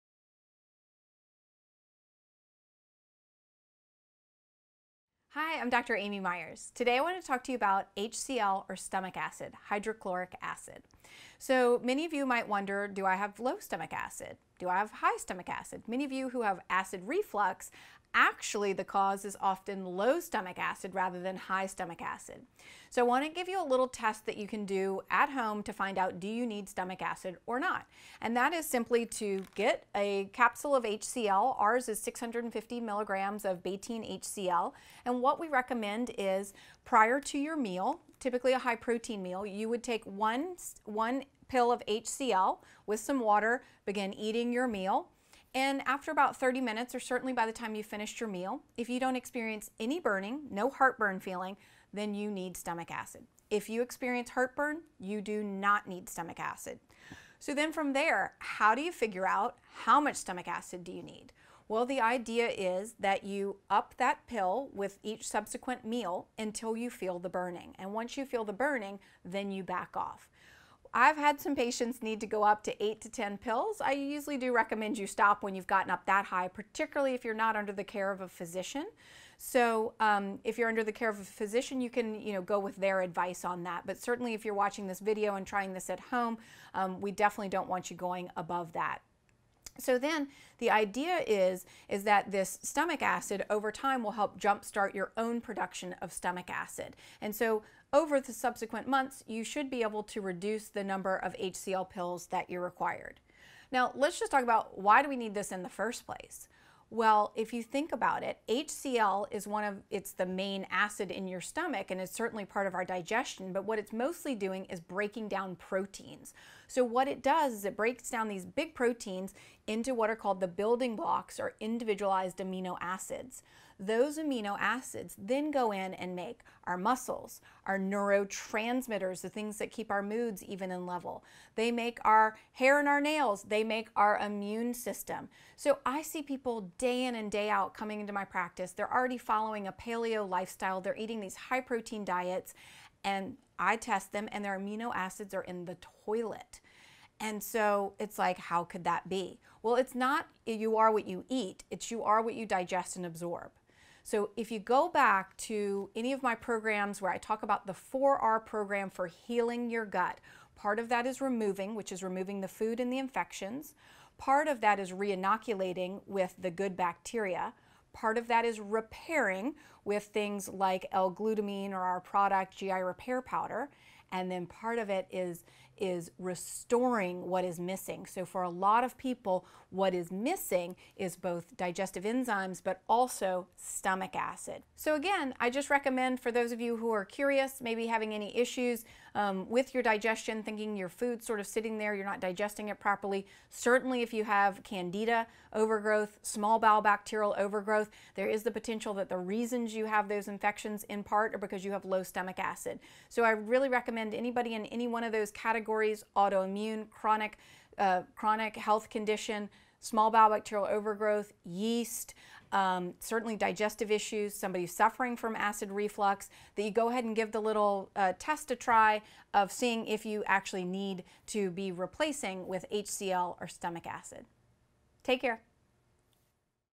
HCL with Betaine Hydrochloride - Do You Need Stomach Acid? | Amy Myers MD®
Stomach acid is key to properly breaking down and digesting your food, yet many people are low on stomach acid. In fact, contrary to popular belief, most people with acid reflux are actually low on stomach acid. Find out how Betaine Hydrochloride can help.